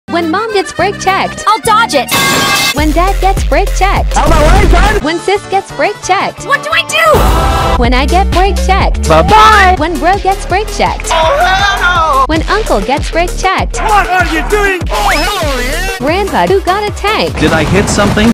When family gets brake checked 😂
*How family - mom and dad drive cars and get brake checked* 😂